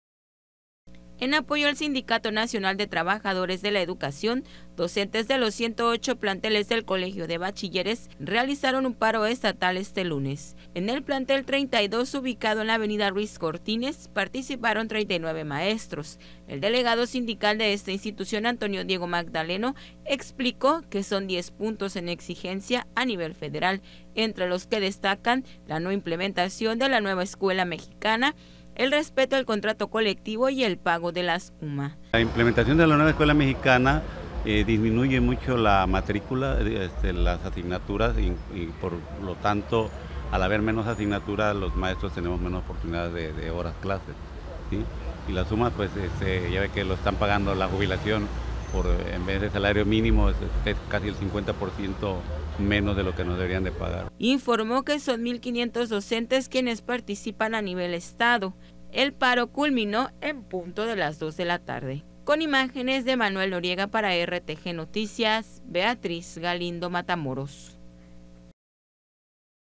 RTGnoticias | 108 planteles de COBACH en Guerrero en paro este lunes
En apoyo al Sindicato Nacional de Trabajadores de la Educación, docentes de los 108 planteles del Colegio de Bachilleres realizaron un paro estatal este lunes.